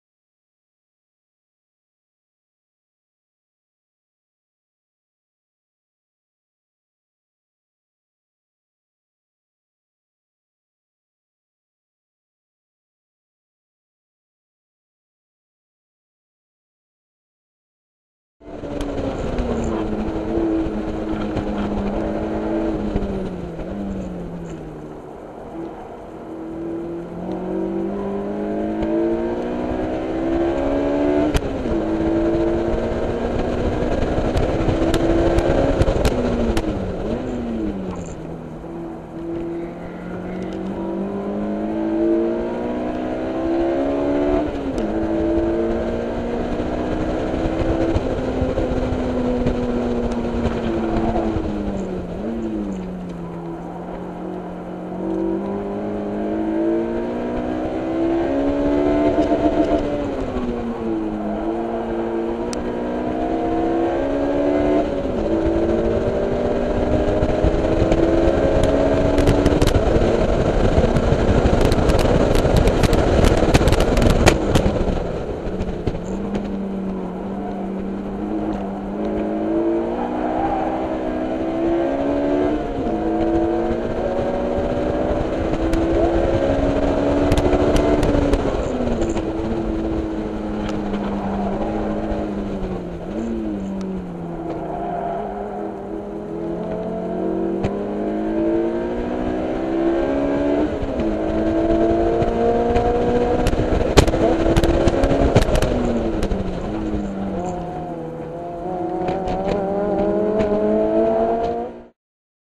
2008 Honda S2000 @ Megara Circuit - 13.12.2012 - PB: 1:09.40
Hellenic Trackdays Open Day
Megara Circuit
13 DEC 2012

The best lap of the day, in a Euro Spec Honda S2000 AP2.

Modifications:
Toyo R888 F225/R255
ACT HS2-HDSS Clutch kit
Fidanza Flywheel
Invidia 60mm Testpipe
EBC Ultimax Grooved Brake Rotors
Hawk DTC-60/HT-10 Brake Pads
Quantum Front Brake Ducts
Sparco PRO2000 Race Seat

Driver:papdoc